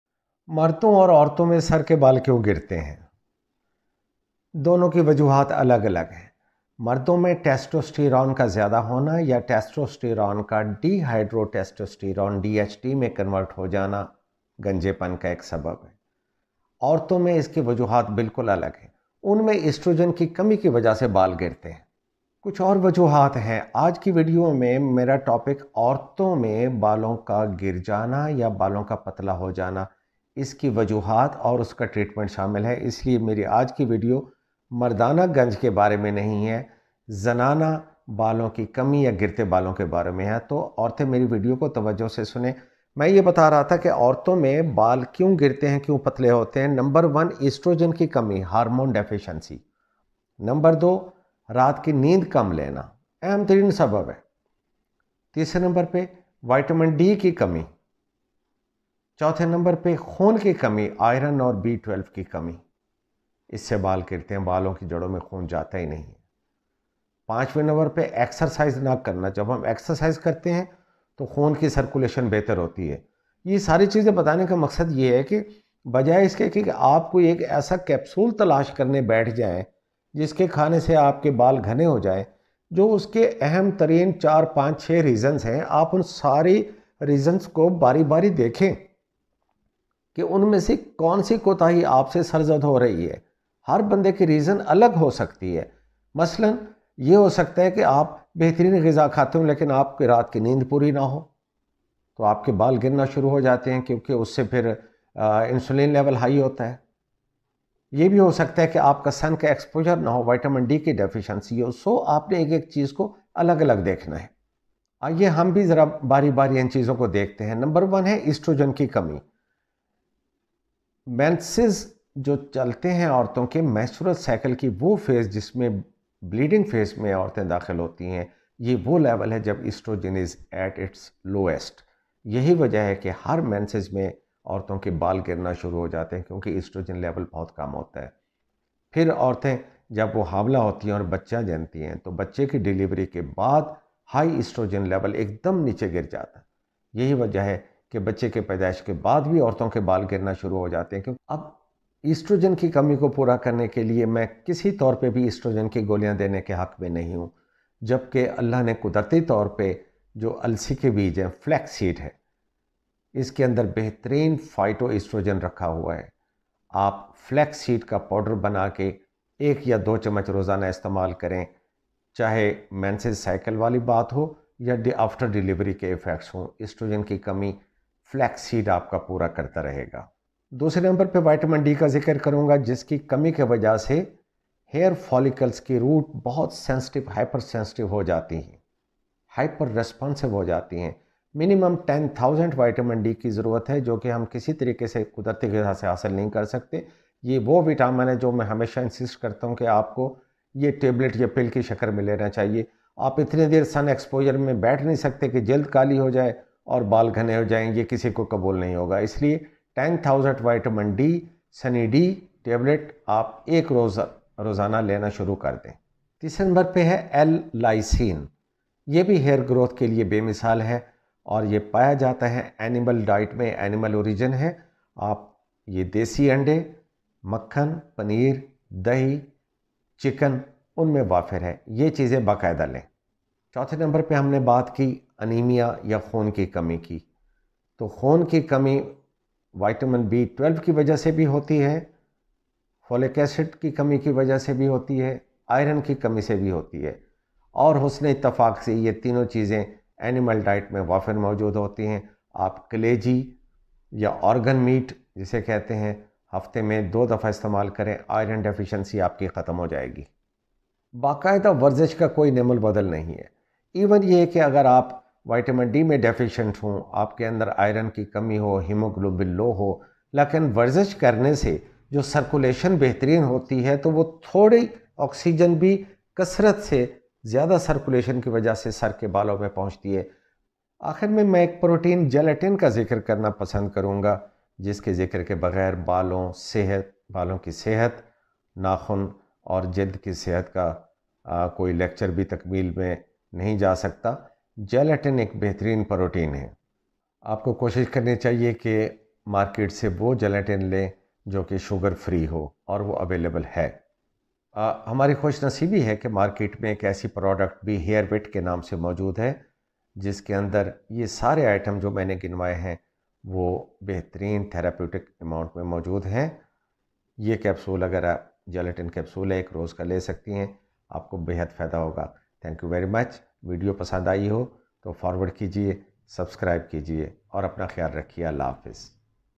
Tips To Stop Hair Fall In Ladies | Girtay balon ka ilaj |Dr Shahzad Hashmi Lecture # 74
Our hair is the crowning glory on
our head. Everyone wants to
know how to stop hair loss and
how we can regrow hair in the
natural way but very importantly,
we need to understand what is
the normal hair loss and what is
abnormal hair loss, which hair
loss can be treated at home
naturally and which hair loss
needs to be seen by a
dermatologist. The first thing is
everybody loses hair around 50
to 100 hairs is normal per day.
But if we already started getting
Scalp thinning or if you are feeling that ur hair loss is more than it, ur concerns are valid.
In my lecture,pls note different reasons of hair fall in ladies and select which remedy suits you,becaz there is no singke panacea that fits all.treatment is individualised.
Gelatin is available in market.